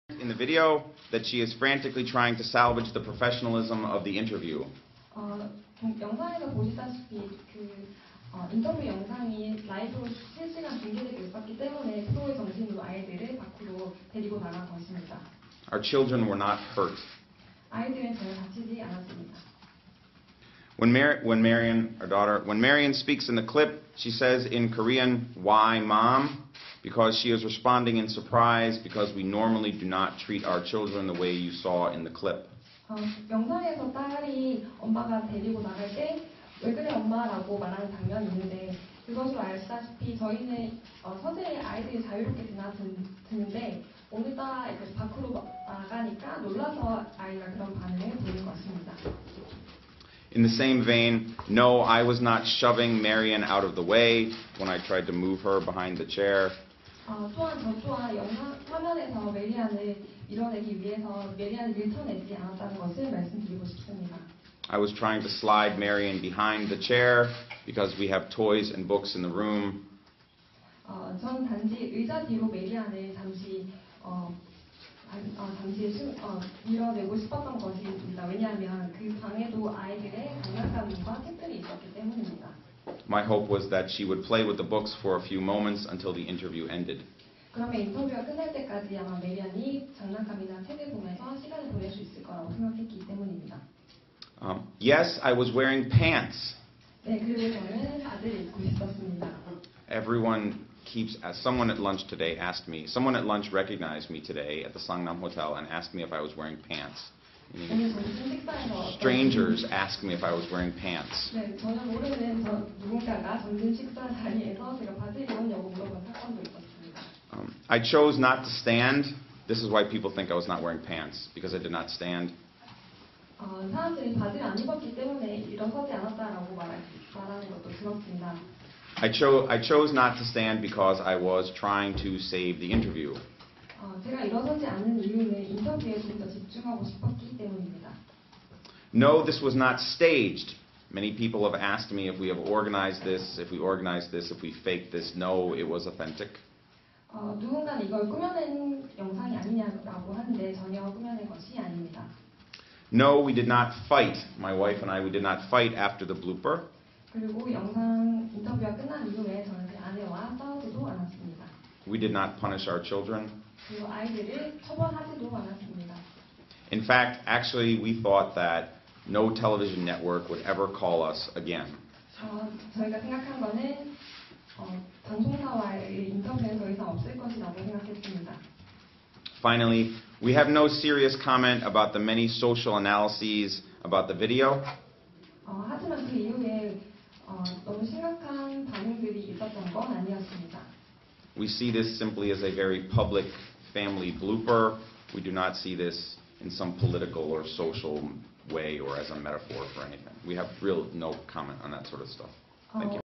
BBC expert Robert Kelly whose family hilariously crashed his interview hold press conference
Daughter Marion is utterly adorable!